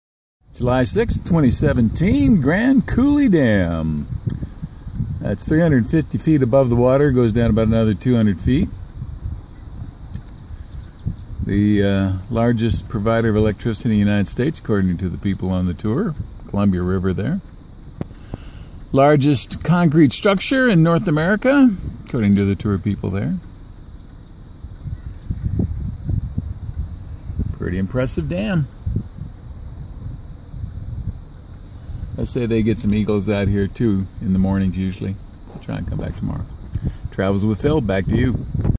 Grand Coulee Dam Washington #2 The Side - July 6, 2017 - Travels With Phil - Unedited
Grand Coulee Dam #2 The Side. This is a look at the dam and the lake from the side of Grand Coulee Dam. The dam is the largest concrete structure in North America, and the 2nd largest in the world. It produced massive amounts of hydroelectric energy. It also helps to control floods and provide irrigation.  - July 6, 2017 - Travels With Phil   by Phil Konstantin - -